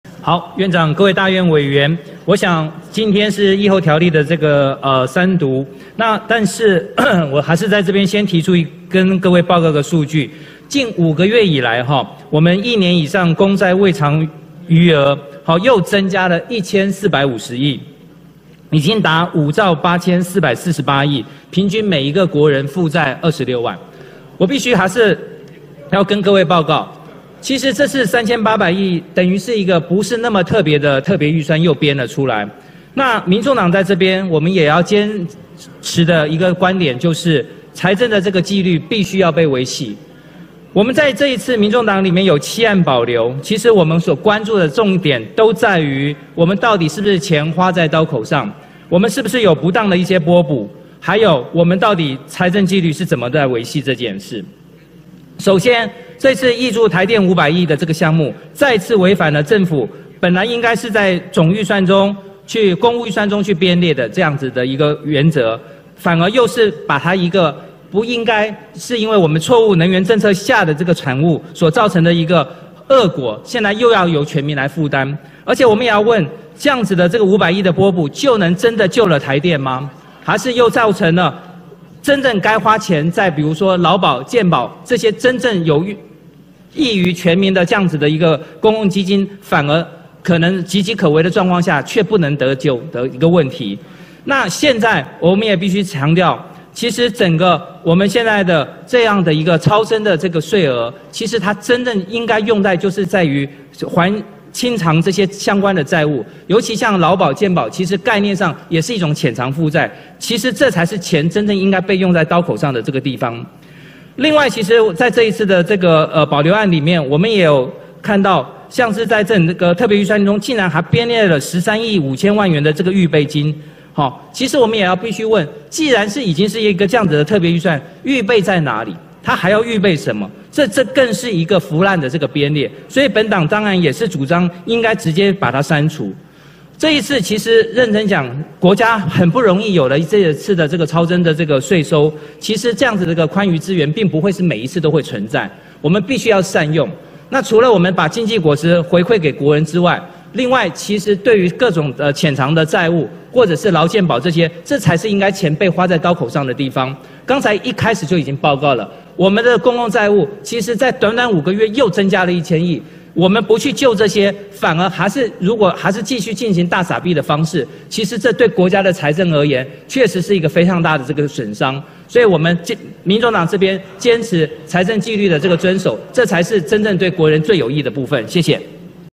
潛藏債務飆高，一年以上公債未償餘額增加了1450億。 已經達5兆8448億，平均每一個國人負債26萬。特別預算中，竟然還編列了13億5000萬元的這個預備金，國家沒有財政紀律對國家是一個非常大損傷
立法委員
💥邀您響應！重要連署💥